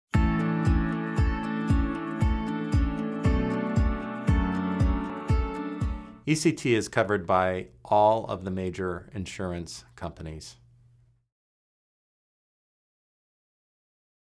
Does Insurance Cover ECT?
ECT, or electroconvulsive therapy, is a psychiatric treatment that has been used for decades. But does insurance cover ECT?

This video takes a look at the different types of insurance coverage for ECT and provides some insight into how people can get this expensive treatment paid for.